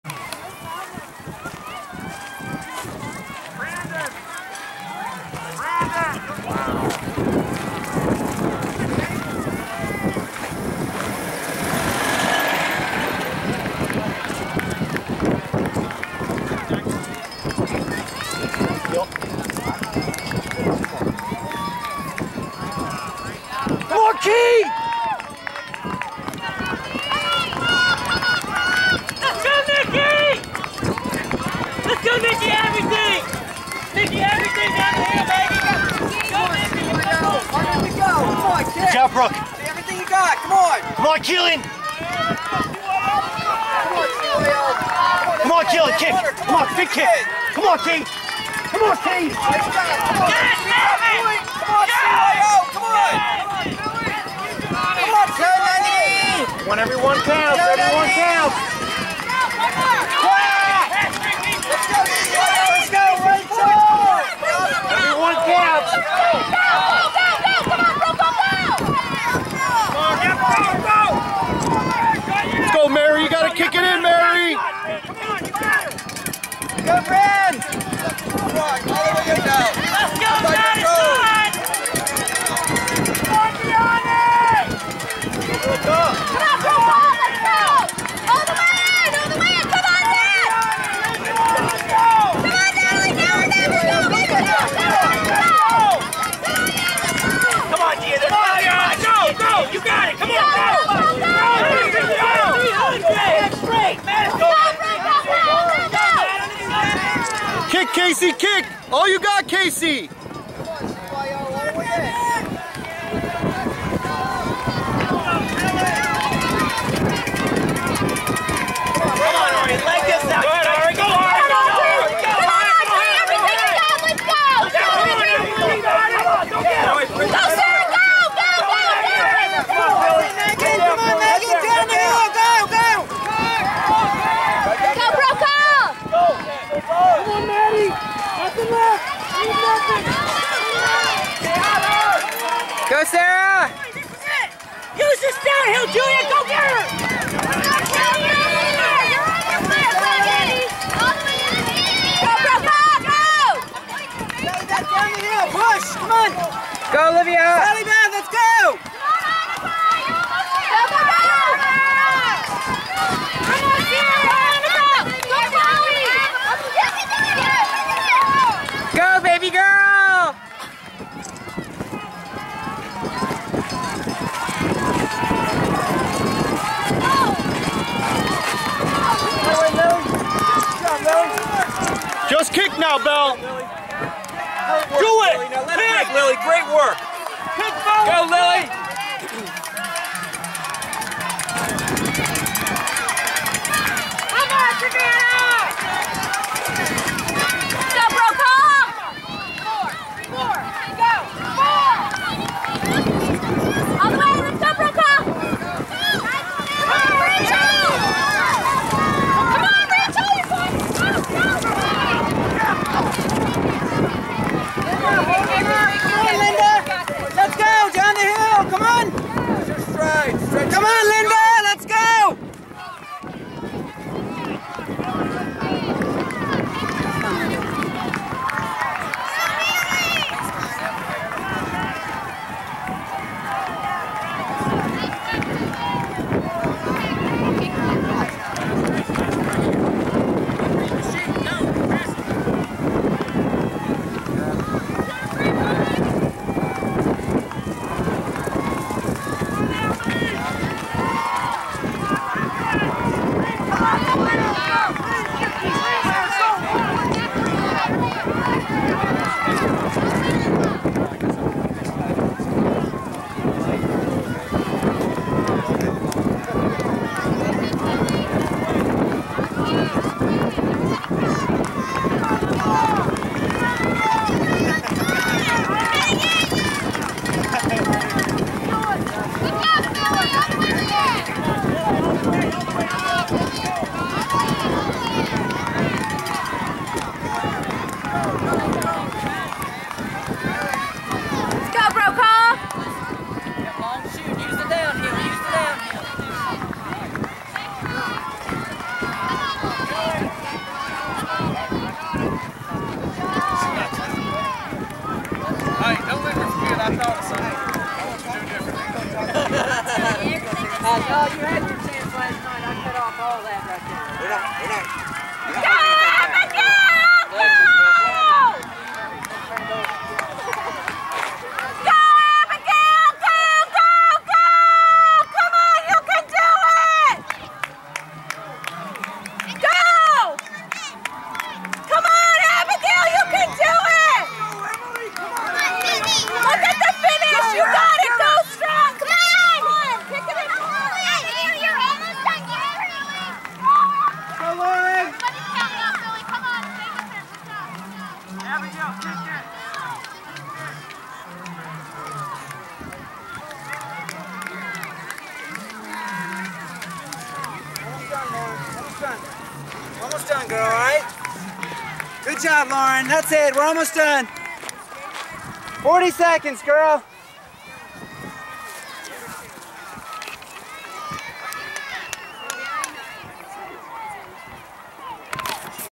Girls 13 14 2
November 22, 2014 Cross Country Coaches National Youth Championships at Percy Warner Park in Nashville, TN.